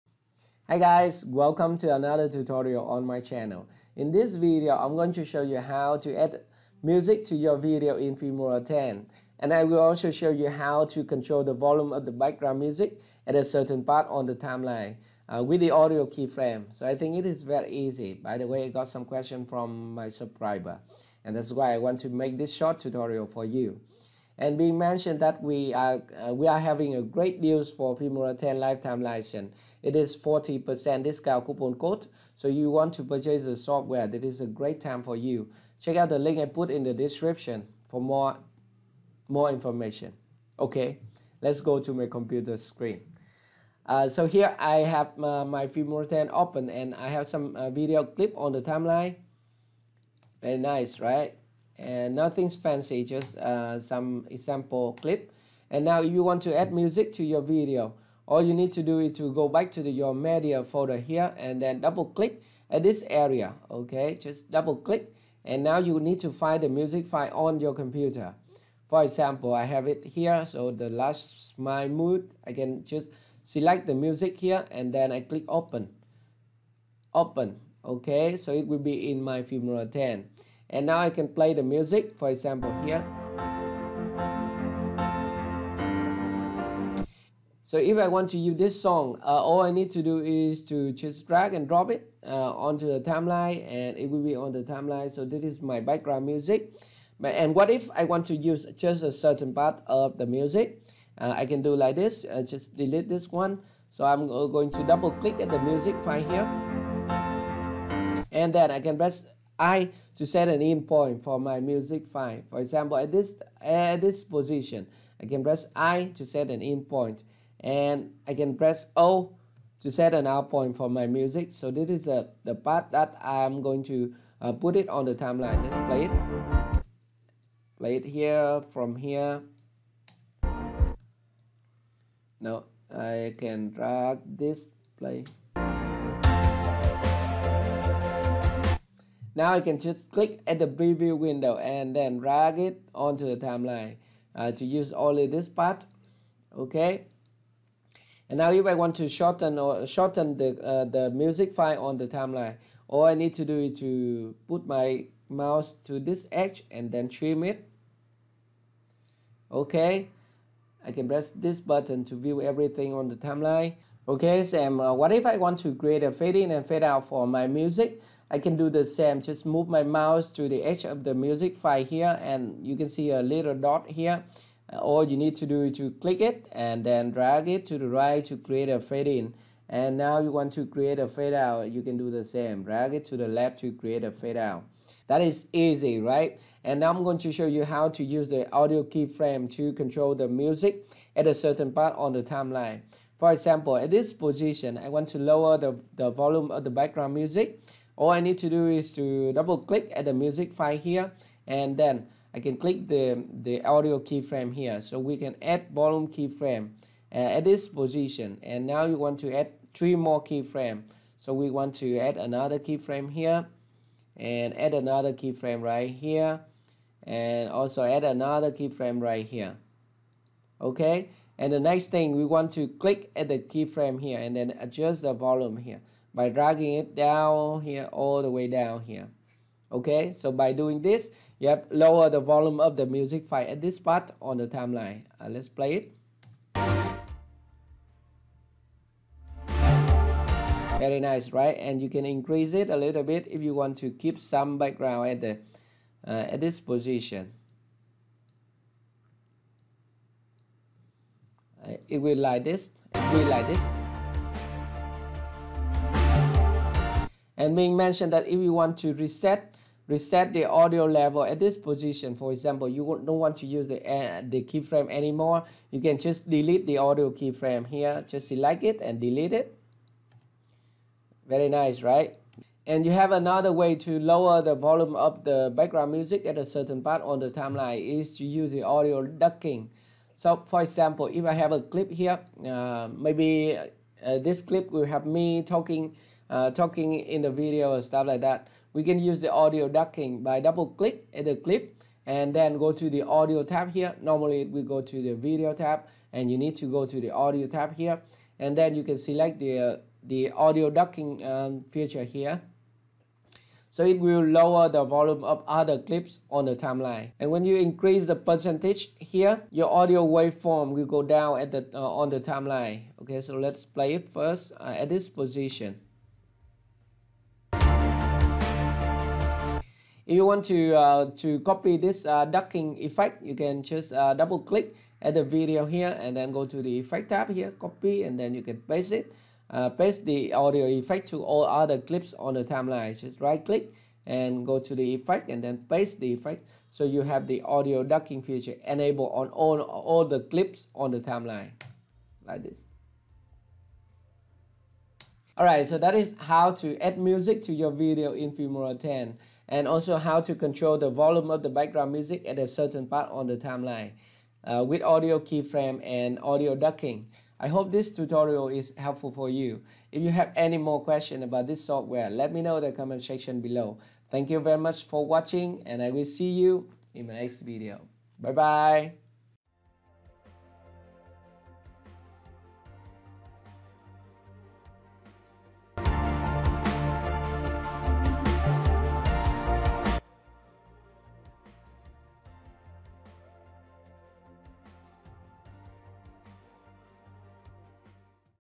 How to add Music in Filmora X
This tutorial shows you how to add music in Filmora X as well as how to use the audio keyframe to control the volume of a certain part of the background music.